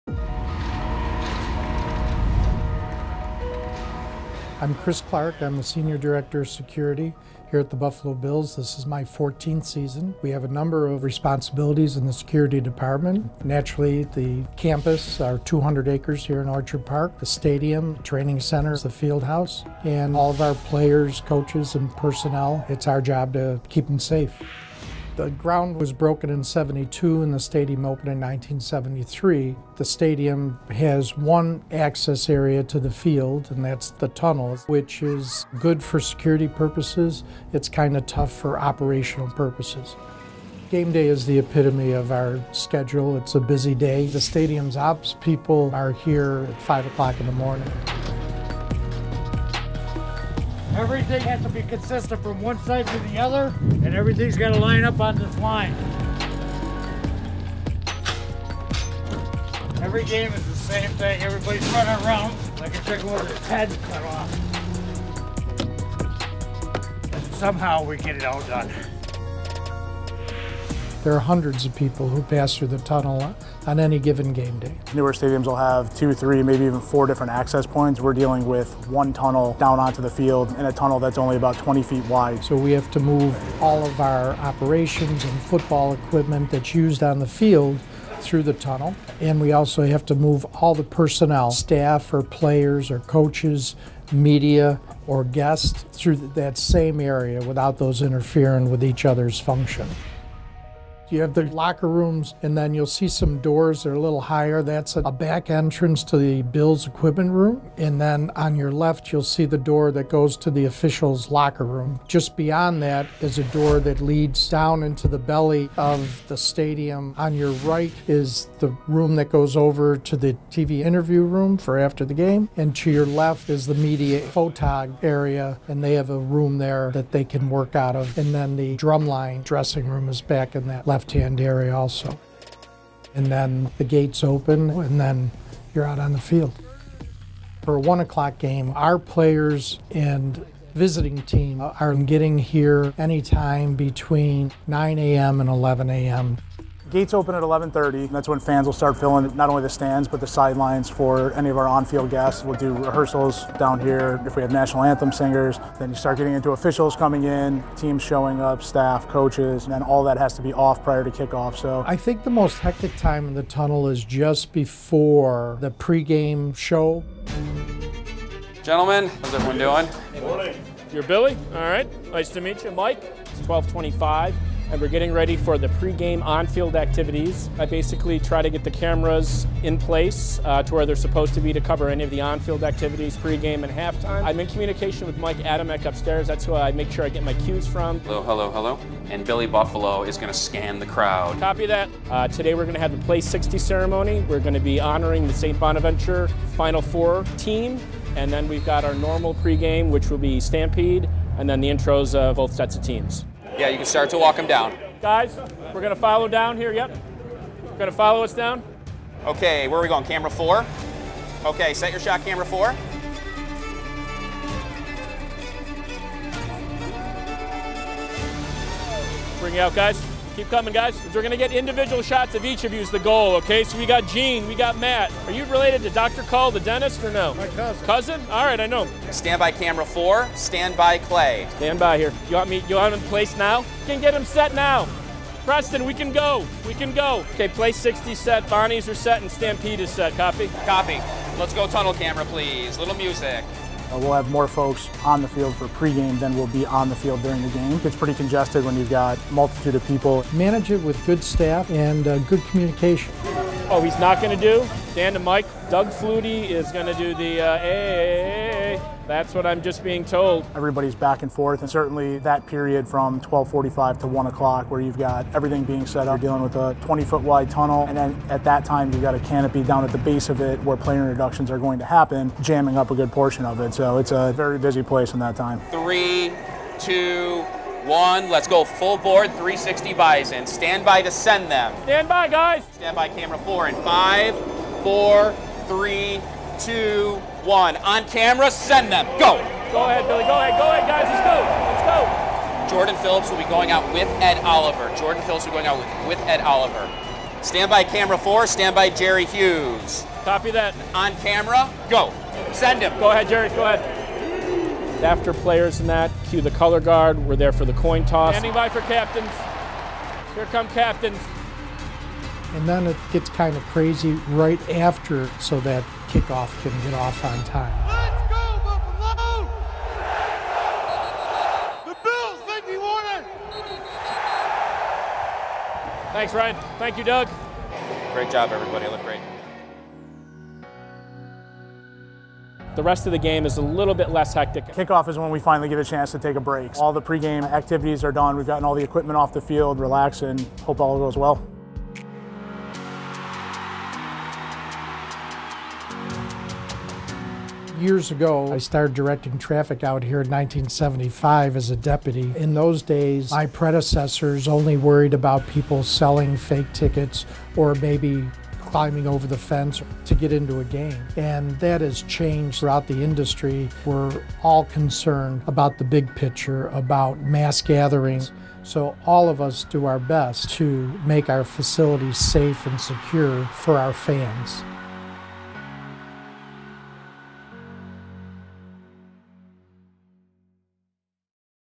Behind the Scenes in the Buffalo Bills' Tunnel
Check out what happens behind the scenes in the Buffalo Bills' tunnel before kick off on game days at New Era Field presented by Nextiva.

Follow: Instagram.com/buffalobills